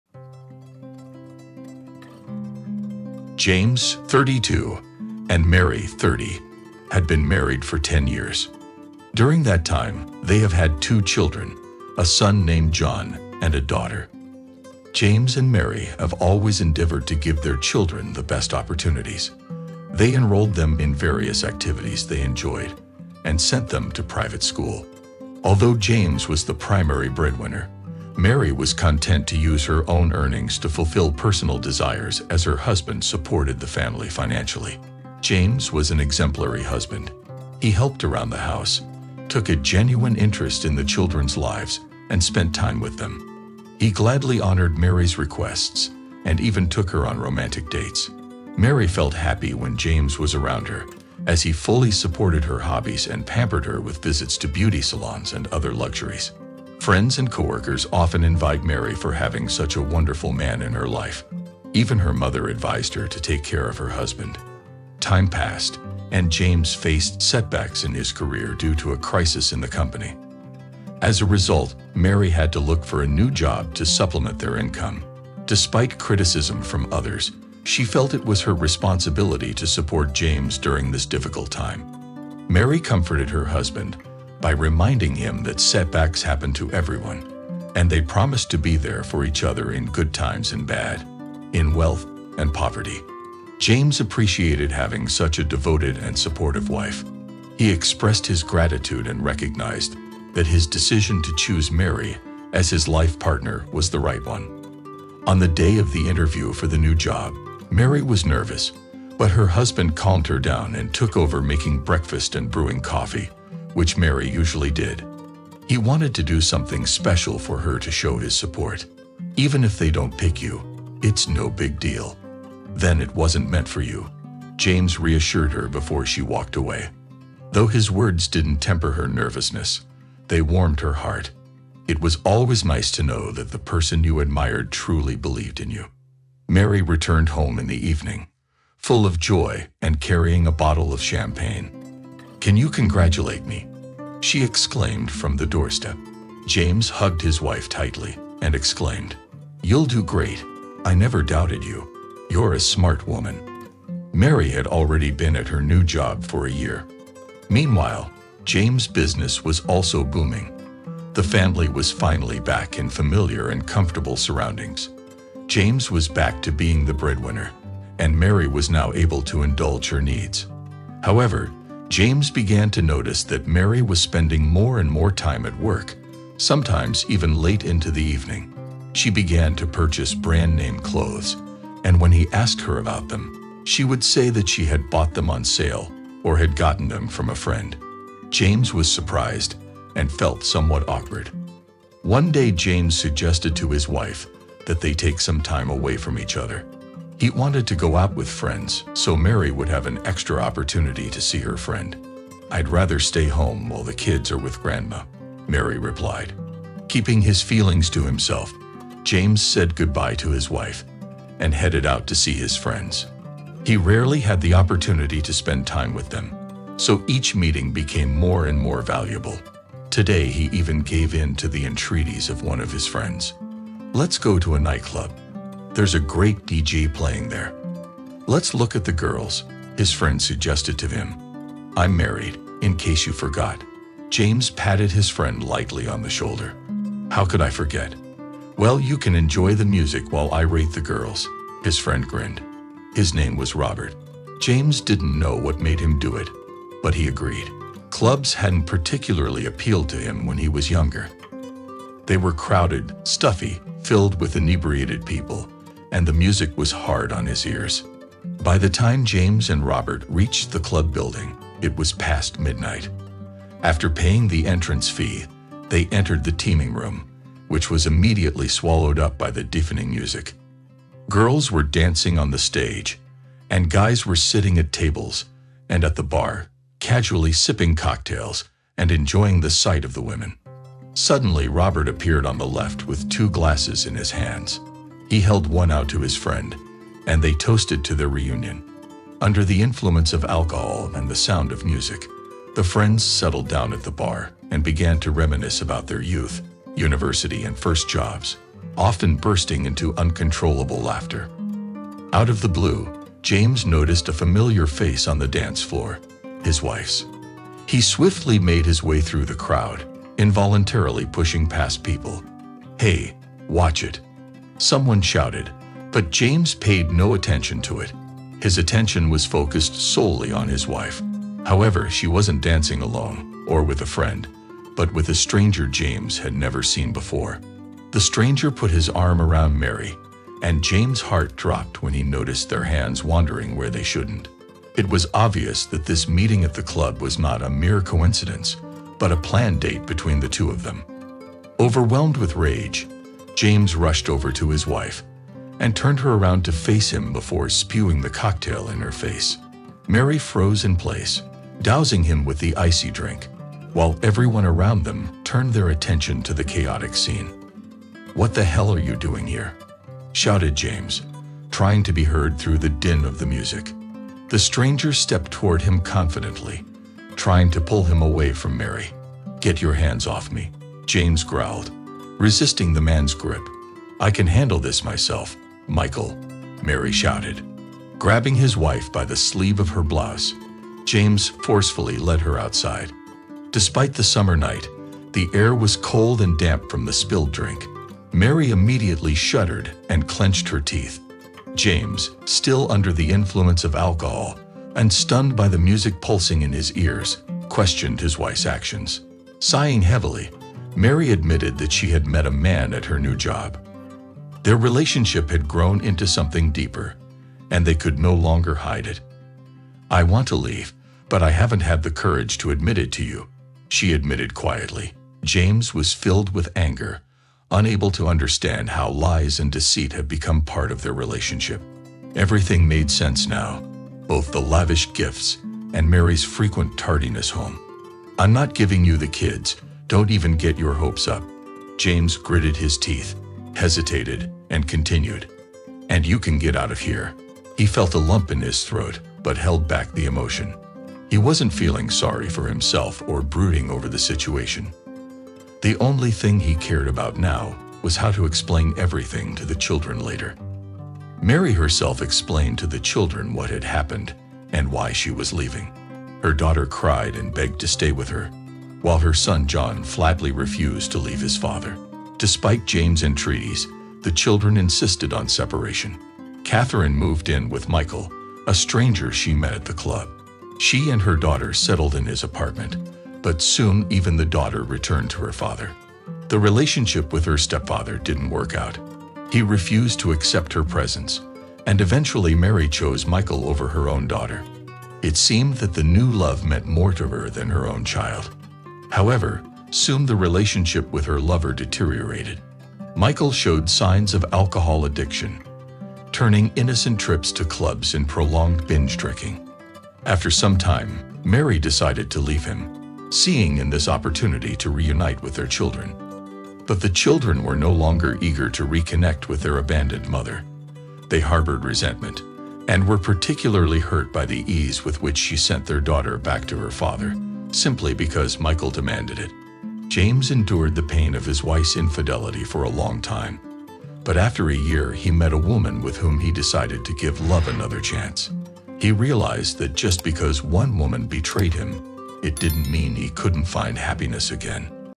I Love My Wife And Support Her In Everything I Worked Hard To Support My Family And One Day...
I Love My Wife And Support Her In Everything I Worked Hard To Support My Family And One Day Reluctantly Agreed To Go To A Club With A Friend I Couldn't Believe My Eyes When I Saw In The Bar...